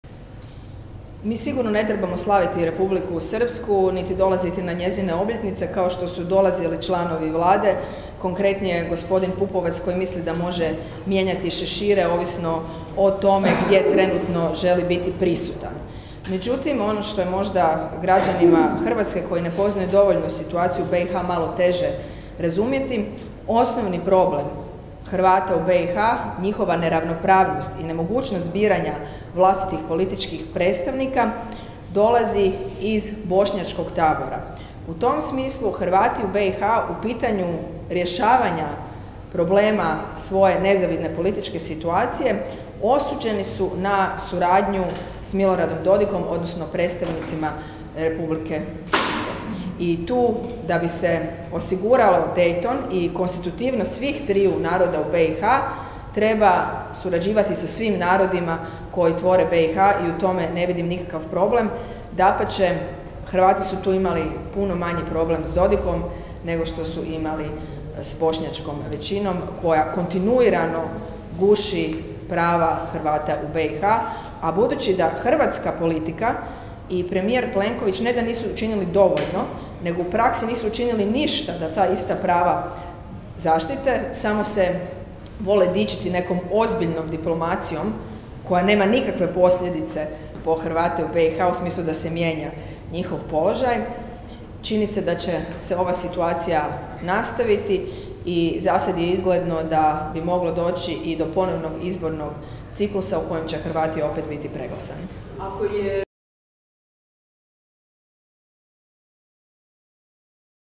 Marija Selak Raspudić o položaju Hrvata u BiH
Na konferenciji za medije Mosta održanoj u Saboru o aktualnim događanjima govorila je zastupnica Marija Selak Raspudić.